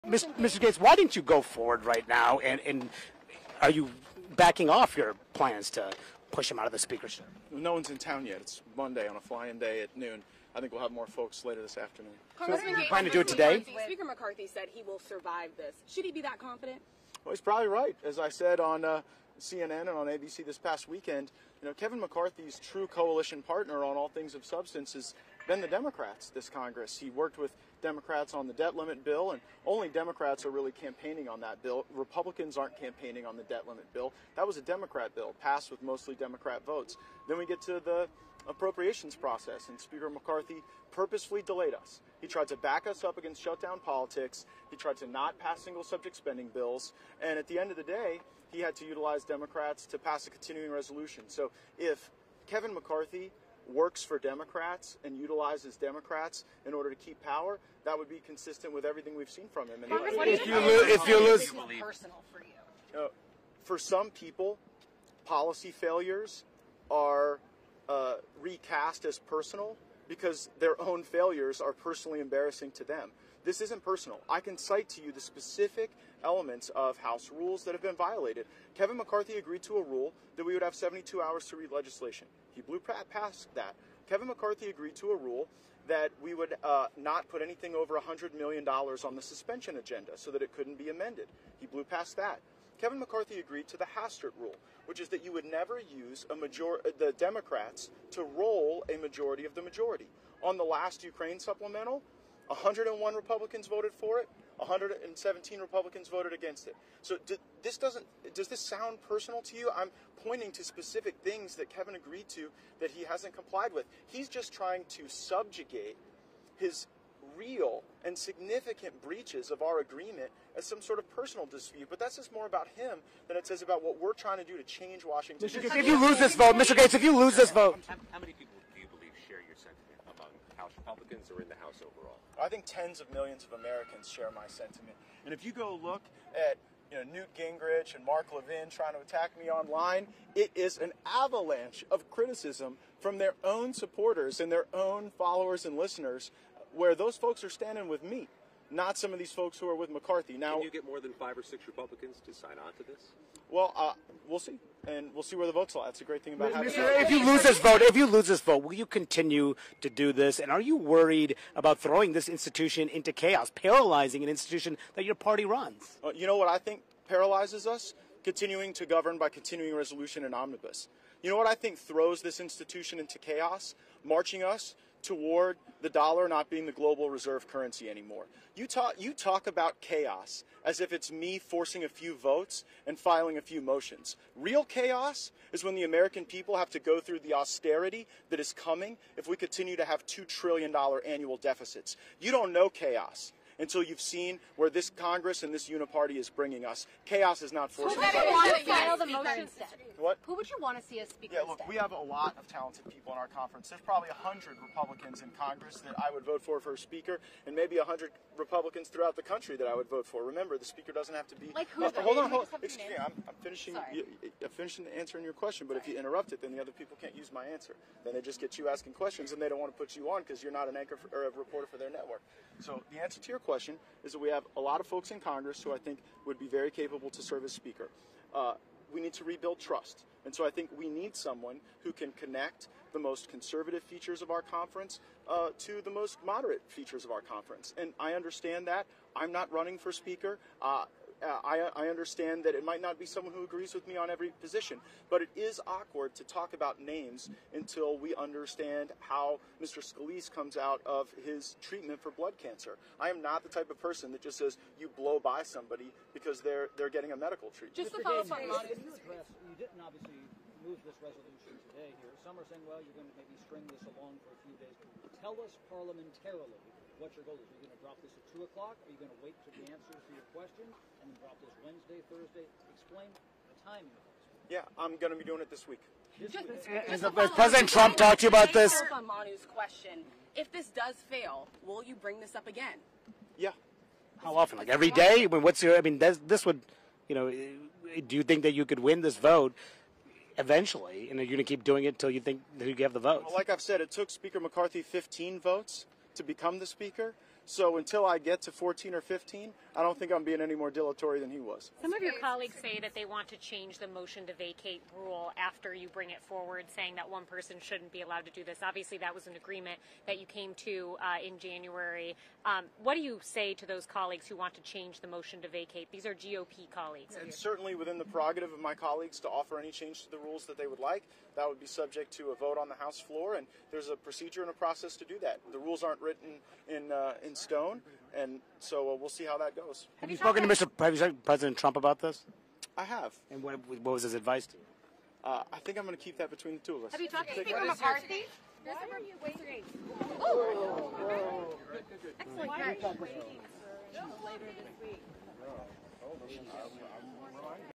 RAW: Rep. Matt Gaetz responds after comments against Speaker McCarthy
Rep. Matt Gaetz spoke on the floor of the House of Representatives today after threatening to call for the ouster of House Speaker McCarthy for working with Democrats to pass a bill that kept the government from shutting down over the weekend.